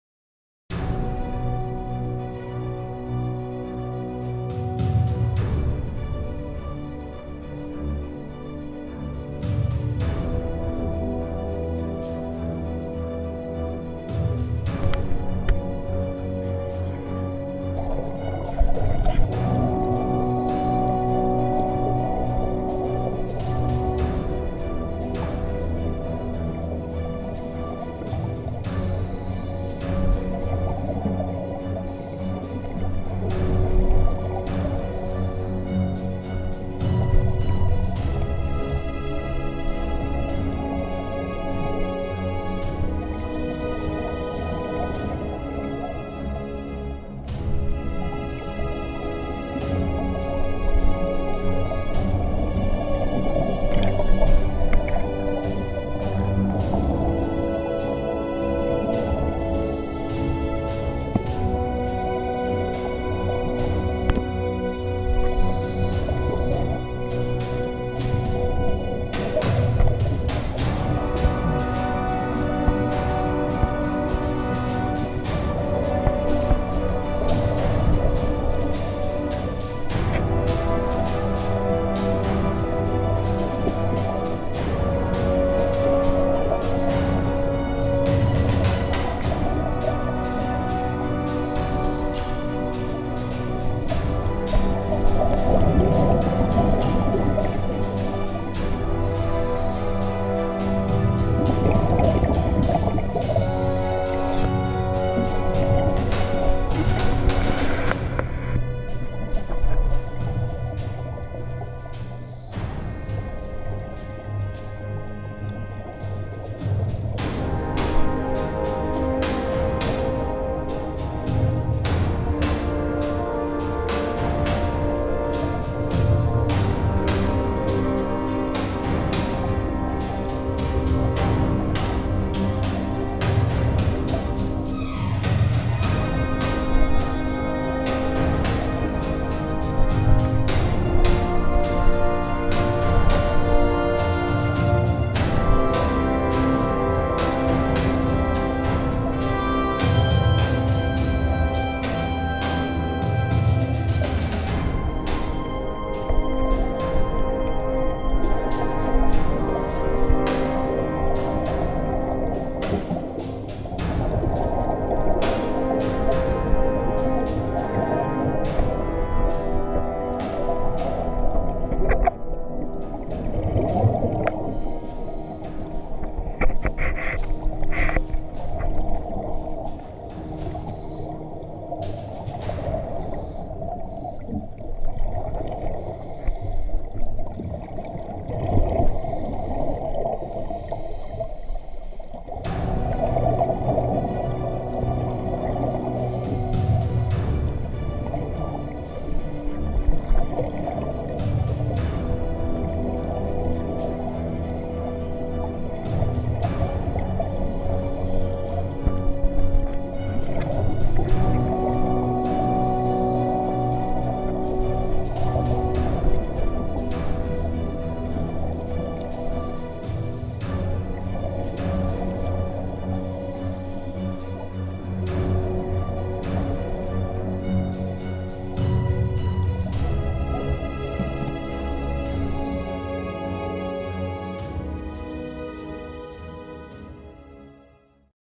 Lucas at the Devil's Den in FL
We need o go back to the pool and practice. Definitely a little rusty